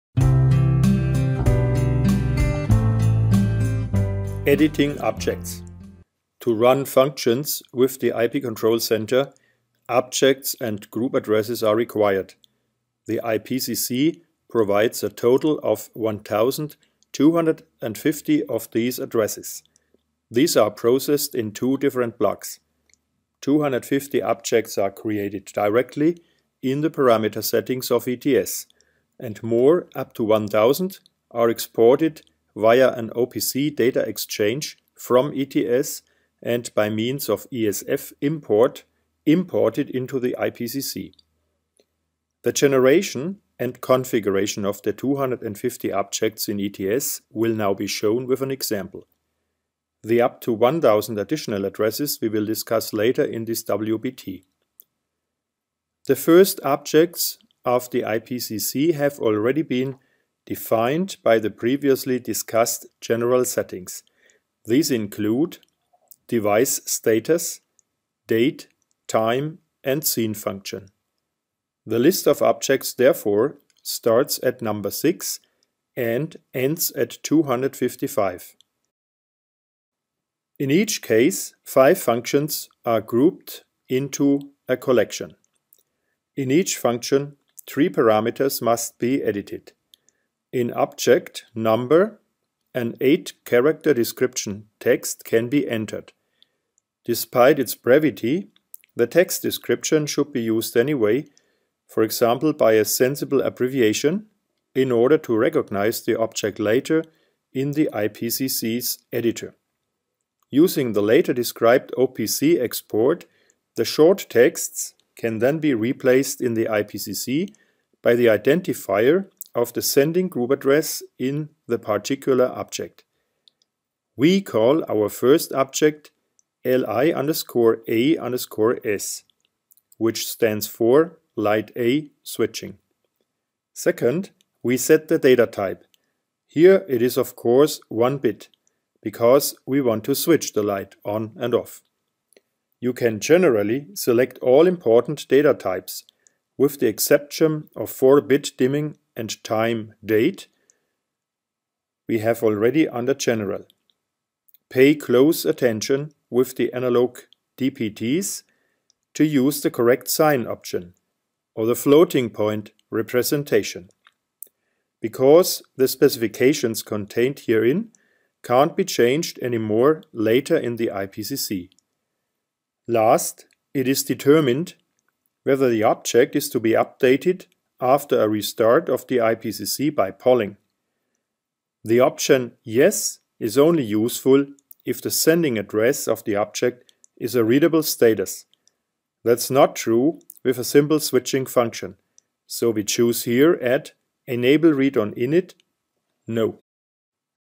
E-Learning Siemens IPCC N152 Part 1 Ch 2 Editing Objects
E-Learning Video for Siemens KNX IP Control Center N152 - Part 1 Chapter 2 - Editing objects in ETS